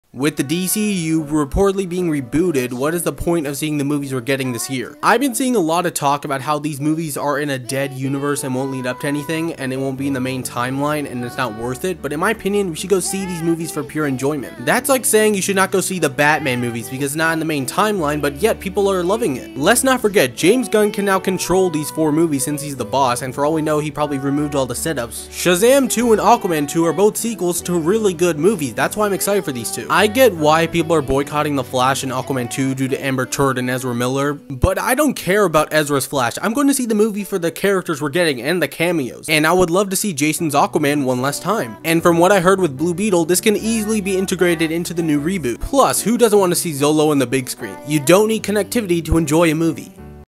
Are The 2023 DC Movies Worth Seeing?
In this video, Sid defends The 2023 DCEU Movies!